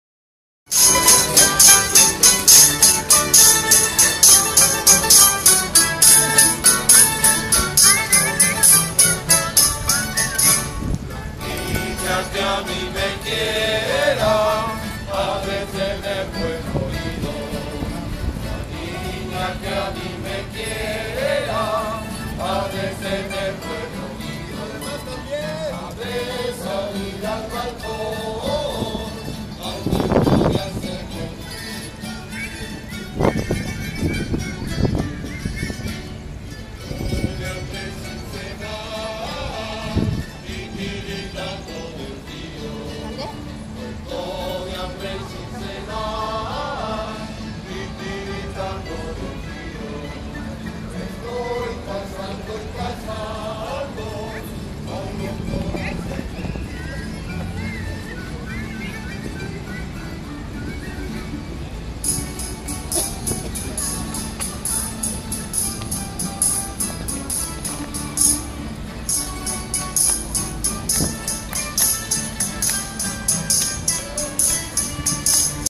Estudiantina en la Plaza de los Fundadores, Irapuato, Guanajuato
Celebración del paseo mágico por el centro de la ciudad de Irapuato. Paso por la plaza de los fundadores.